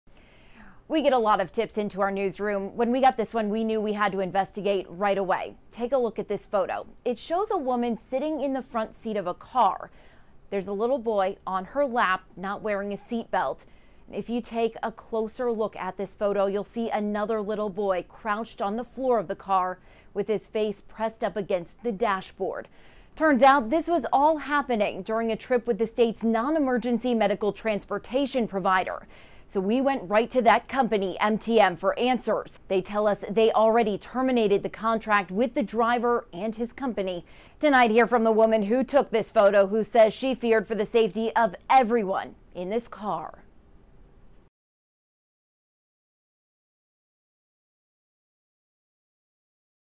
VIDEO NOW: Susan Campbell explains photo of a dangerous drive
A Target 12 Exclusive coming up at 5.